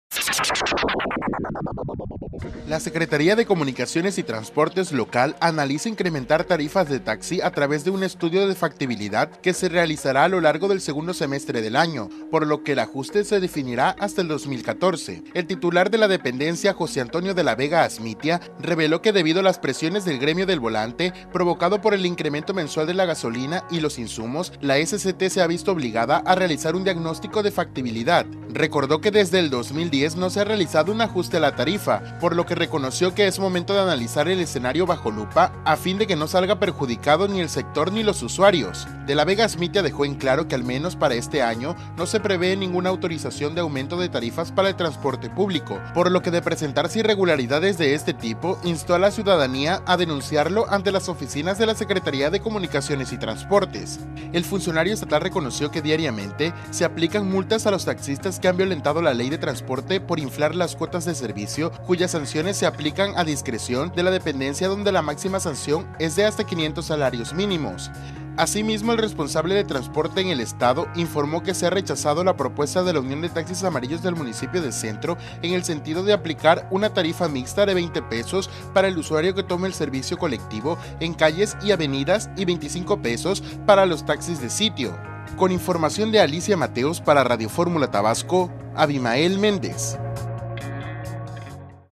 No habrá aumento en las tarifas de taxis hasta 2014: SCT Tabasco
Las Secretaría de Comunicaciones y Transportes local analiza incrementar tarifas de taxi a través de un estudio de factibilidad que se realizará a lo largo del segundo semestre del año, por lo que el  ajuste se definirá hasta el 2014.

El titular de esta dependencia, José Antonio de la Vega Asmitia, reveló que debido a las presiones del gremio del volante, provocado por el incremento mensual de la gasolina y los insumos, la SCT se ha visto obligada a realizar un diagnóstico de factibilidad.